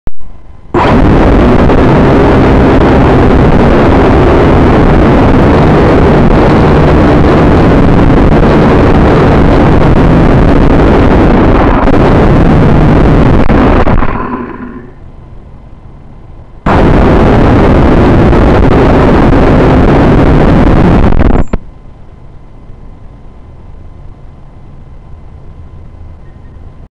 Syma X5C Outdoor Flight 1
Captured myself on film before flying up and then making a hard landing.